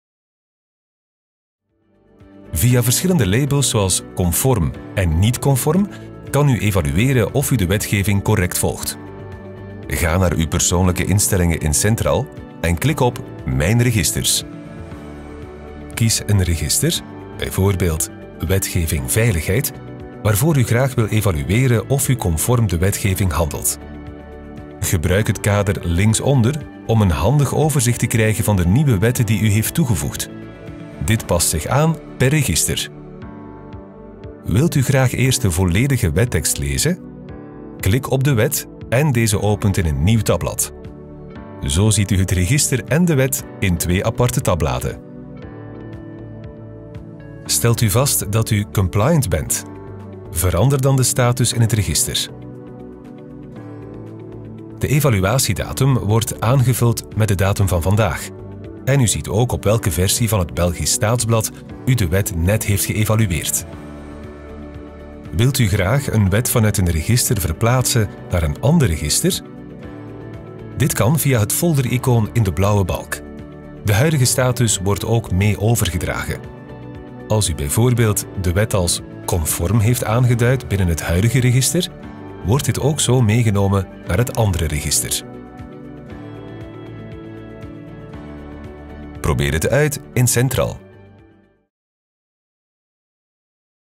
senTRAL – wetgevingsregister: geef wetten een status
Wilt u als preventieadviseur graag een beter overzicht over de voor u relevante wetgeving bewaren, eenvoudig bepalen welke acties u dient te ondernemen én focussen op de communicatie naar het management?

Gebruik dan het wetgevingsregister in senTRAL. Eens u wetten heeft toegevoegd aan een register, kan u deze wetten een status geven. Per register kan u de wetten evalueren door de status conform, niet conform of in progress te geven. Zo behoudt u steeds het overzicht en mist u geen enkele wetswijziging.